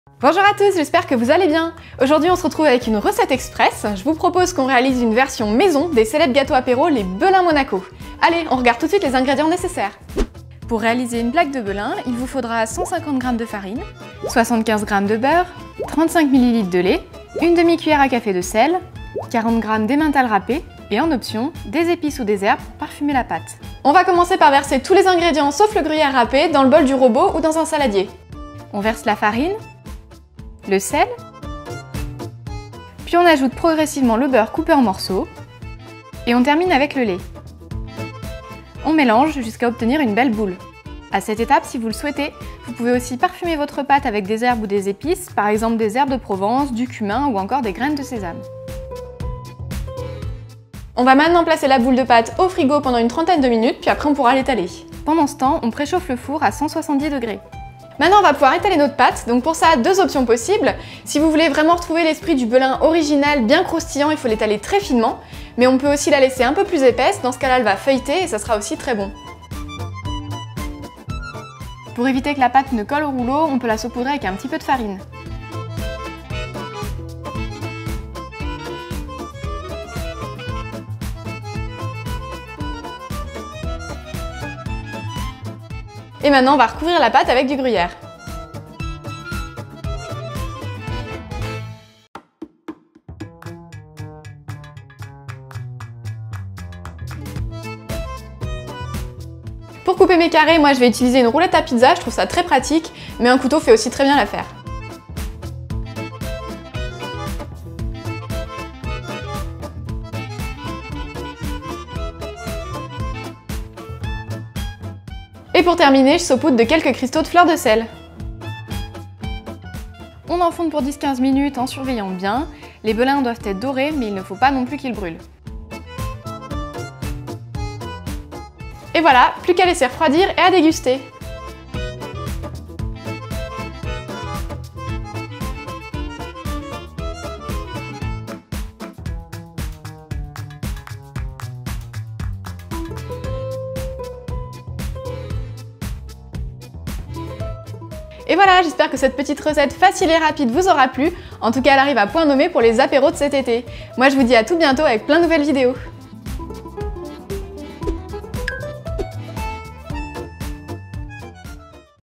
Recette apéro - Belins faits maison
Aujourd'hui je vous propose une recette super facile pour reproduire la fameux gâteaux apéro "Belins Monaco"

► INGREDIENTS
- 150g de farine
- 75g de beurre en morceaux
- 35 ml de lait
- 1/2 car de sel
- 40g d'emmental râpé
- En option : épices ou herbes pour parfumer la pâte

Déroulé de la vidéo :
0:00 Intro
0:10 Ingrédients
0:24 Préparation de la pâte
2:05 Sortie du four
2:24 Résultat final
2:29 Conclusion

Merci beaucoup d'avoir regardé la vidéo, j'espère qu'elle vous a plu, n'hésiter pas à me laisser un commentaire si vous avez la moindre question :)

► Lien de l'article : à venir